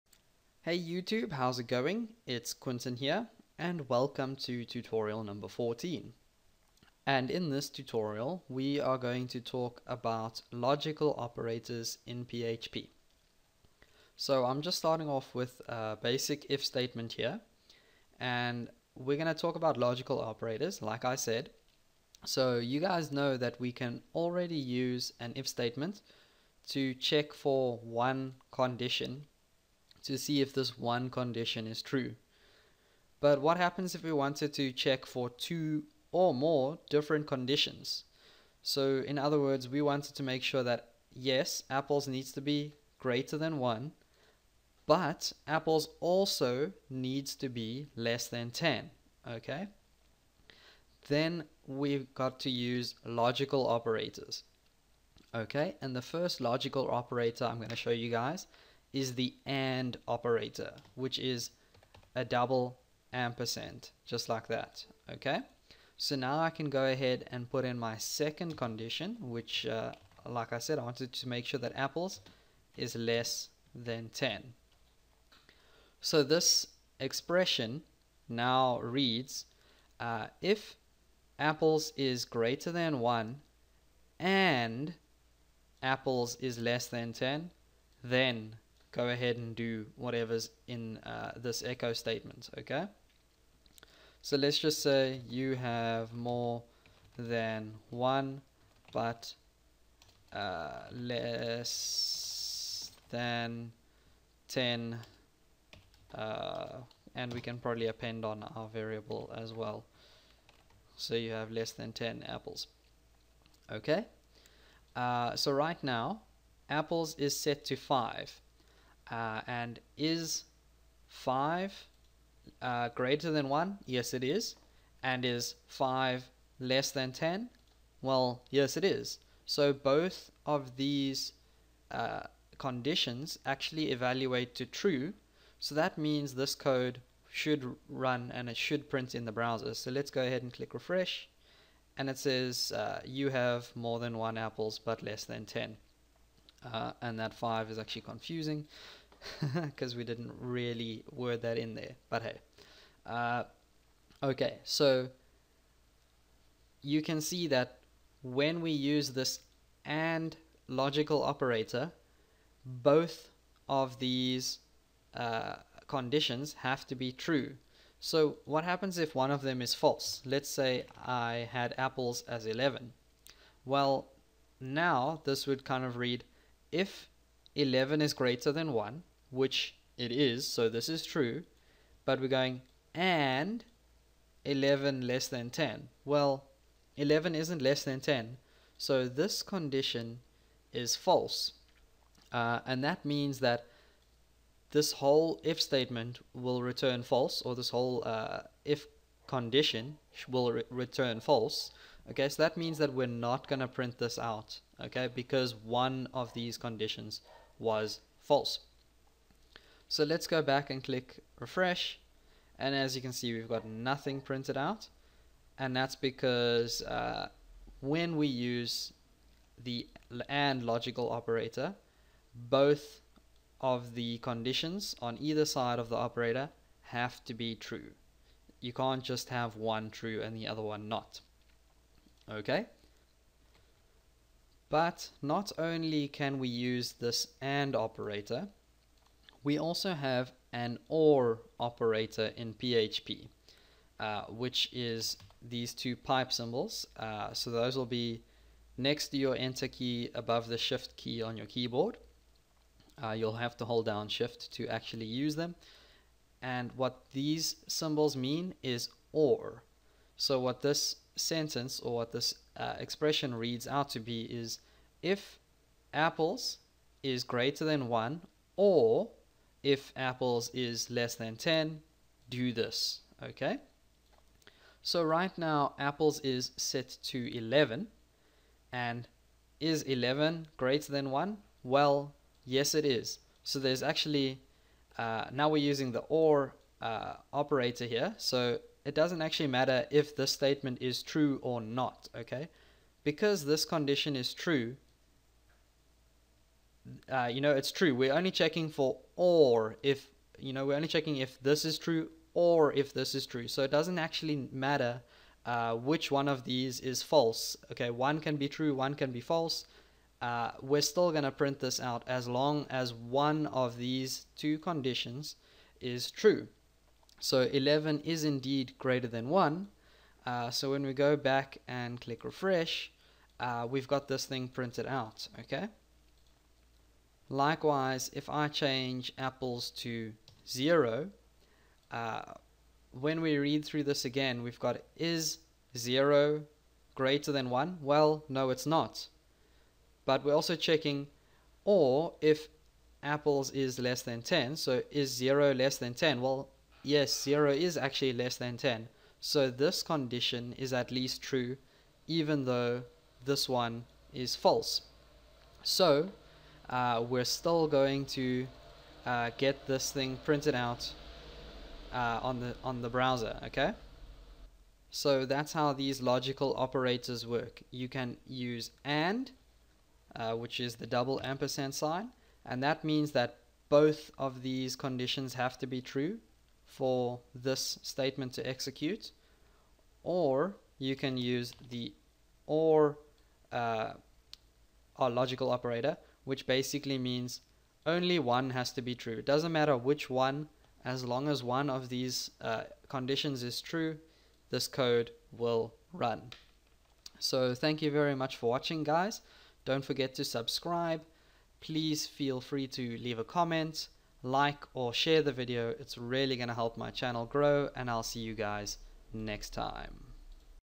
PHP beginners tutorial 14 - logical operators
In this condition I talk about the logical operators in PHP which allow us to check for more than 1 condition at a time.